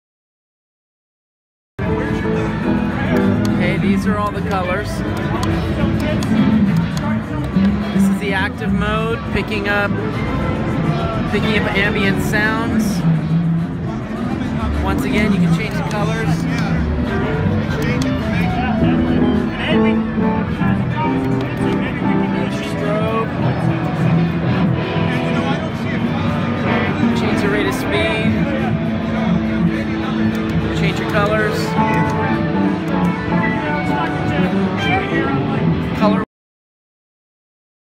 NAMM Oddities 2016 - StompLight
The StompLight personal lighting system as seen at Winter NAMM 2016 and featured on nammoddities.com